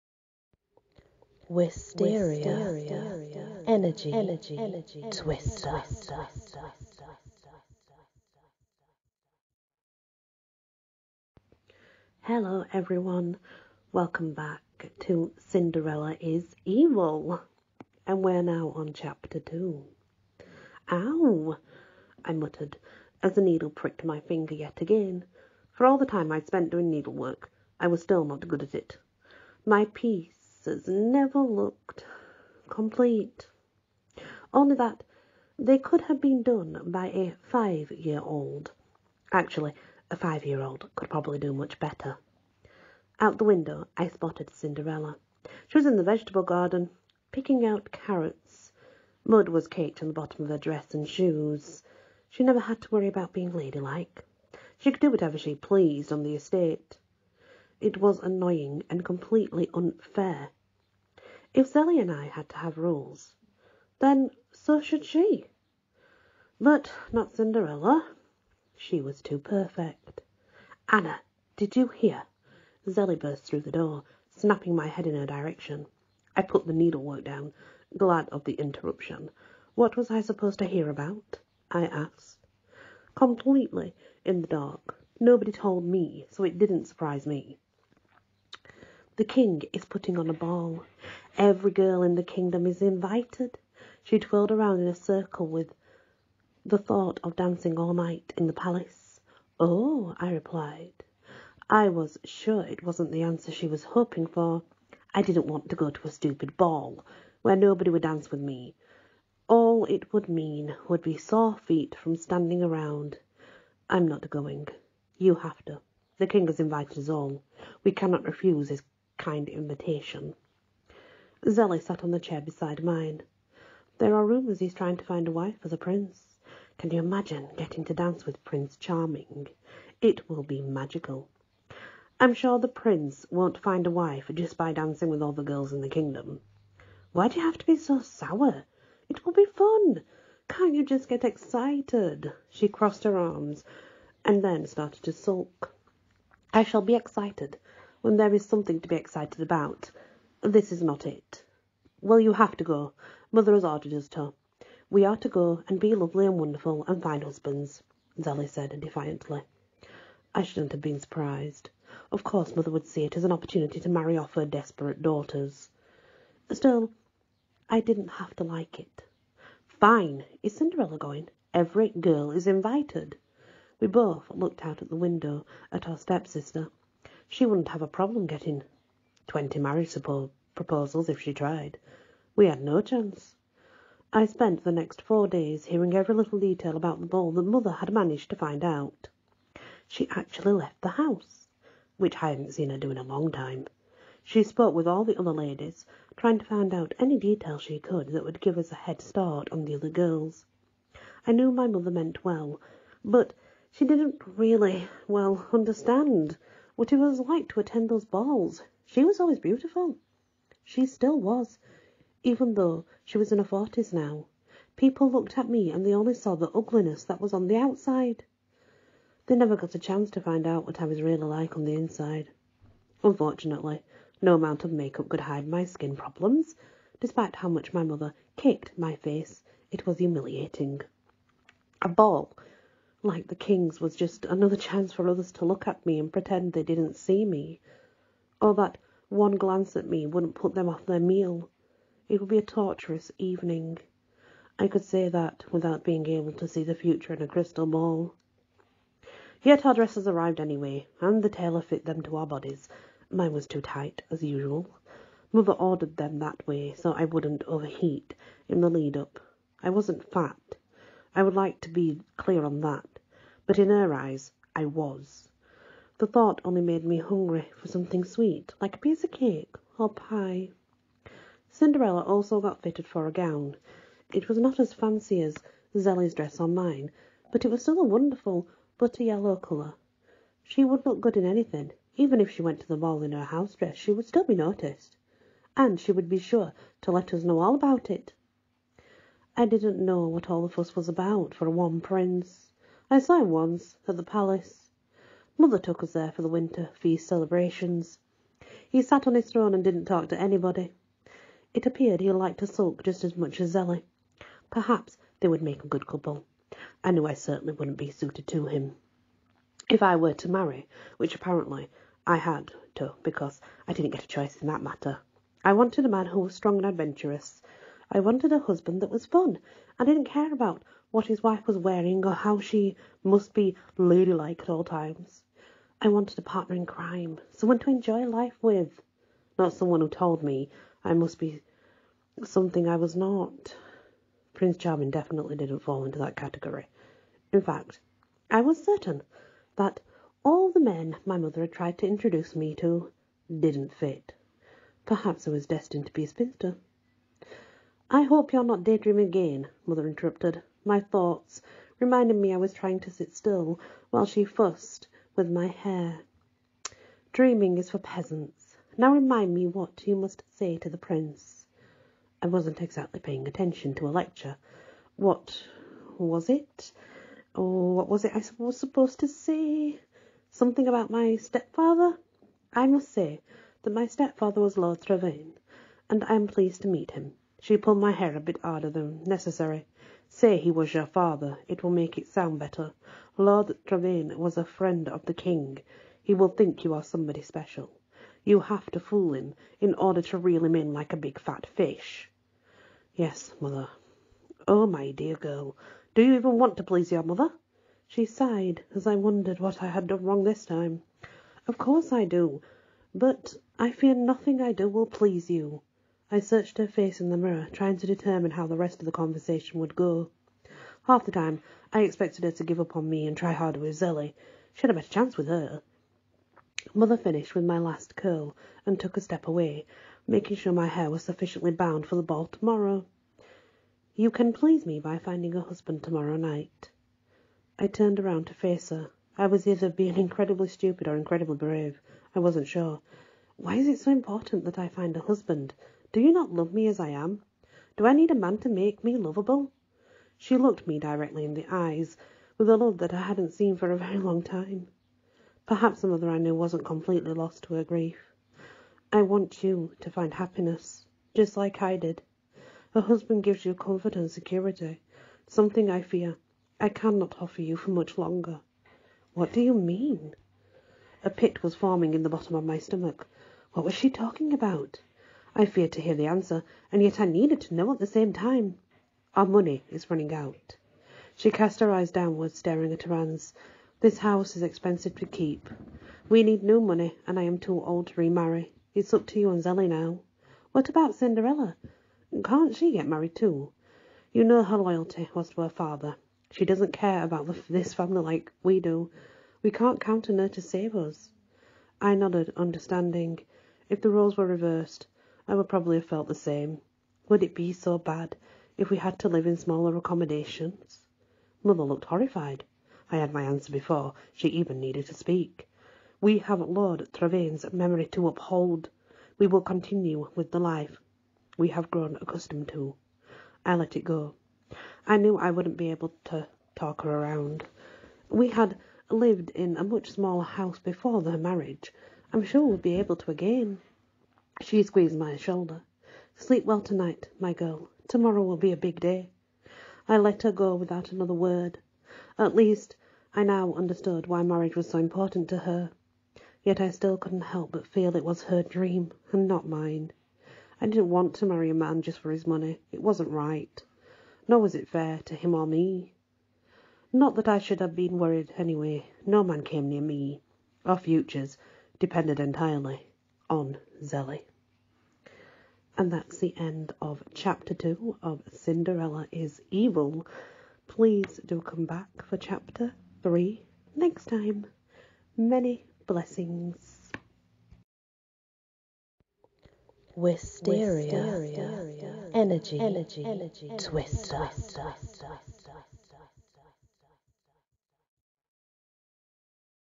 She's too perfect
The story of Cinderella as told by her Step-sister Anna.

Credit to Author : Jamie Campbell

Read by: Wysteria